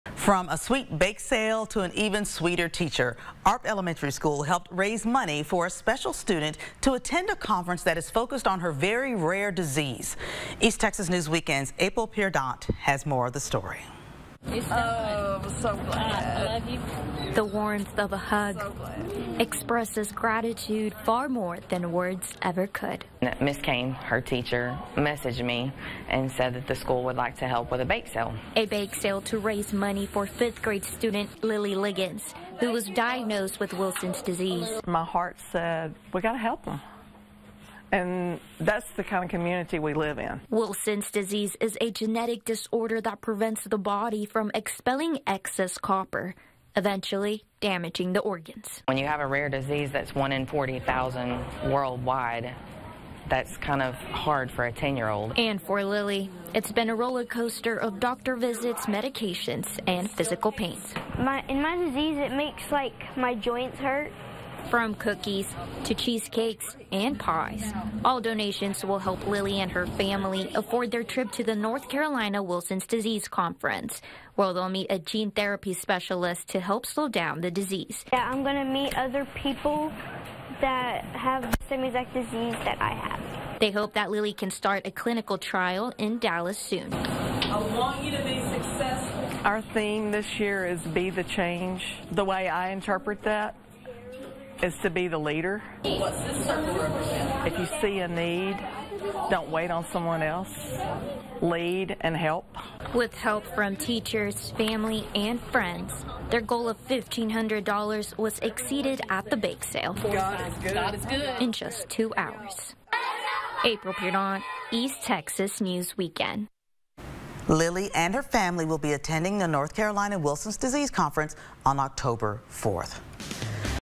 Arp school holds bake sale to support student with rare disease
Arp Elementary School helped raise funds for a special student to attend a conference that's focused on her rare disease.